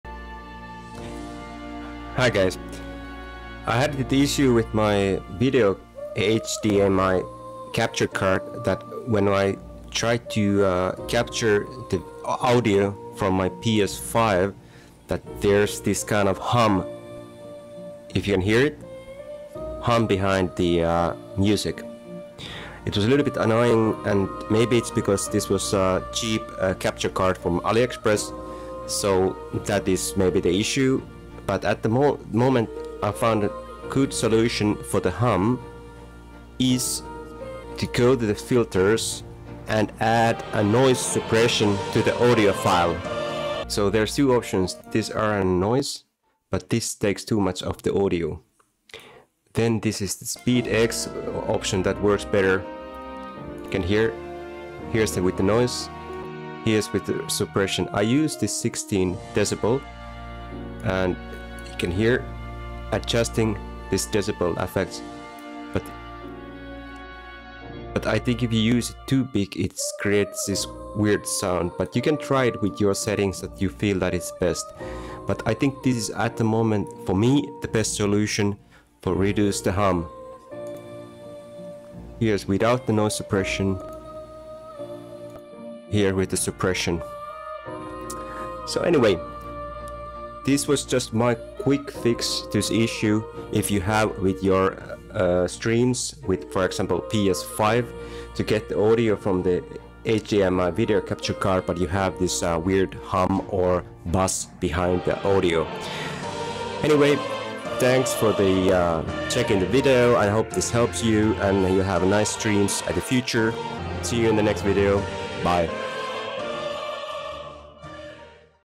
How to Fix PS5 Audio Buzz/Hum with HDMI Video Capture Card in OBS streaming.
Here is a fix for the AUDIO buzz hum static noise when streaming with my HDMI Video Capture card from ALiExpress